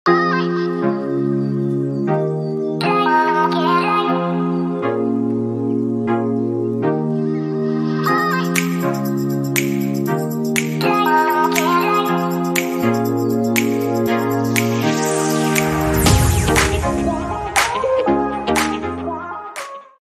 He cleans her face before she gives birth 🥰
Male dog cleans pregnant dogs face and ears just before she gives birth to her puppies 😍 Shop all our favorite pet finds and recommendations

Musician: @iksonmusic